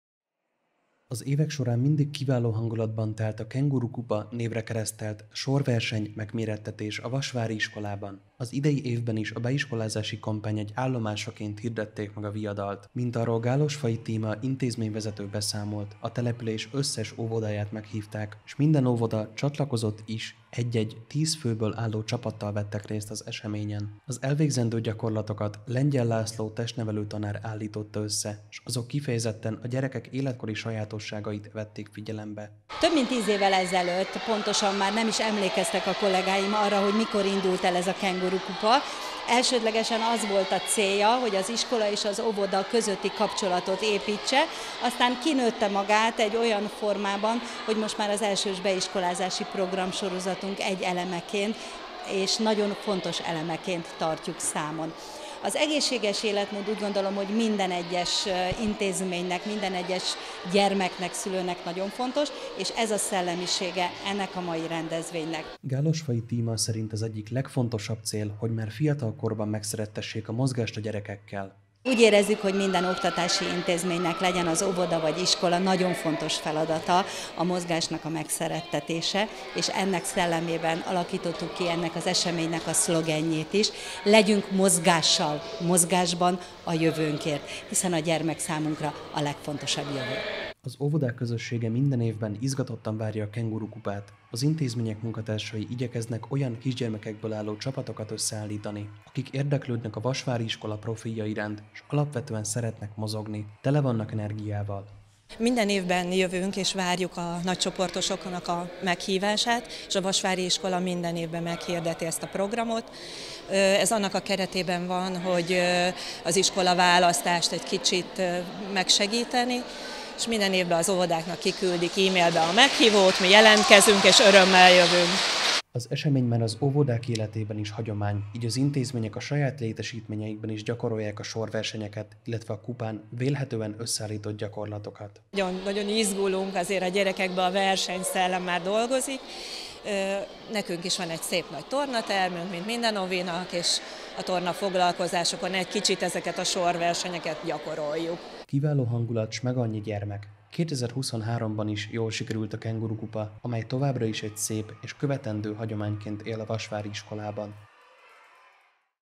DSTV | KENGURU KUPA AZ OVISOKNAK
Egy hosszú évekre visszanyúló hagyomány folytatódott a Vasvári Pál Általános Iskolában. Az intézményben újra megrendezték a Kenguru Kupát, a versenyre pedig a település minden óvodája delegált egy-egy csapatot.